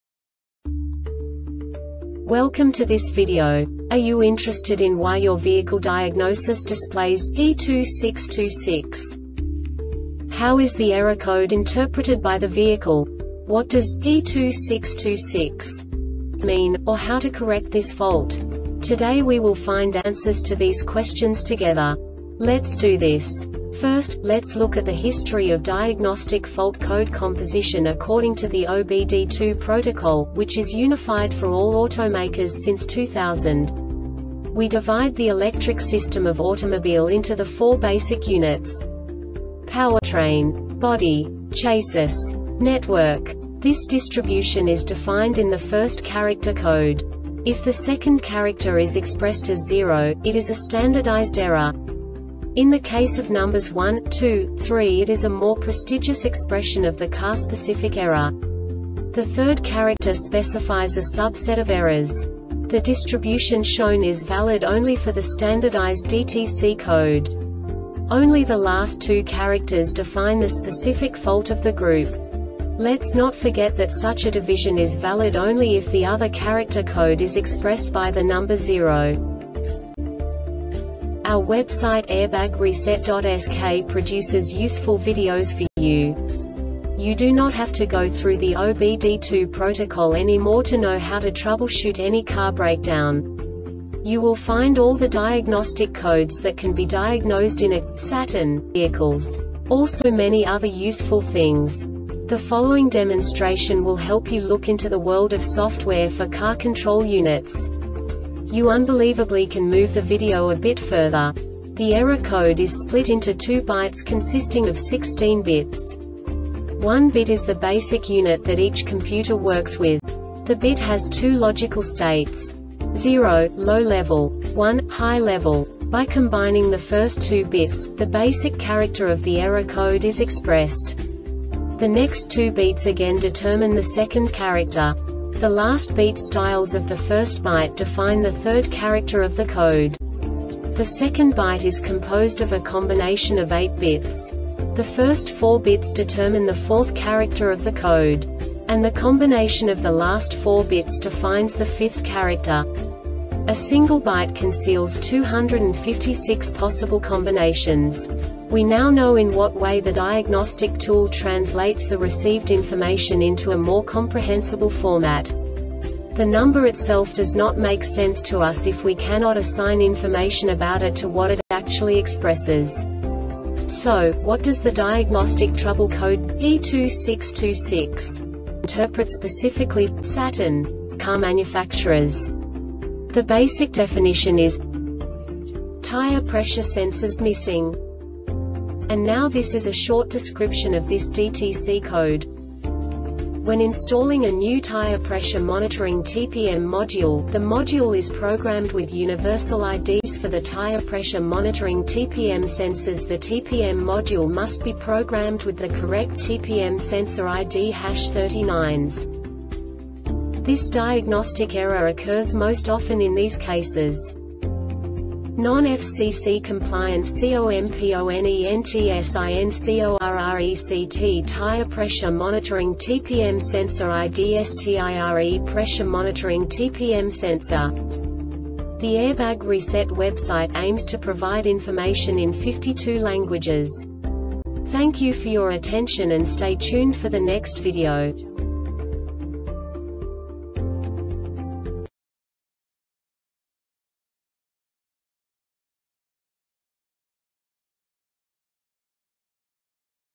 DTC Saturn P2626 Short Explanation
Contents:
0:21 Basic DTC analysis according to OBD2 protocol standard.
1:48 Insight into programming
2:58 A diagnostically understandable description of the Diagnostic Trouble Code
3:05 Basic error definition

Make:
Saturn
Code:
P2626
Definition:
Tire Pressure Sensors Missing
Description:
When installing a new Tire Pressure Monitoring (TPM) Module, the module is programed with universal IDs for the Tire Pressure Monitoring (TPM) Sensors. The TPM Module must be programmed with the correct TPM Sensor ID&#39;s.
Cause:
NON-FCC COMPLIANT COMPONENTSINCORRECT TIRE PRESSURE MONITORING (TPM) SENSOR ID(S)TIRE PRESSURE MONITORING (TPM) SENSOR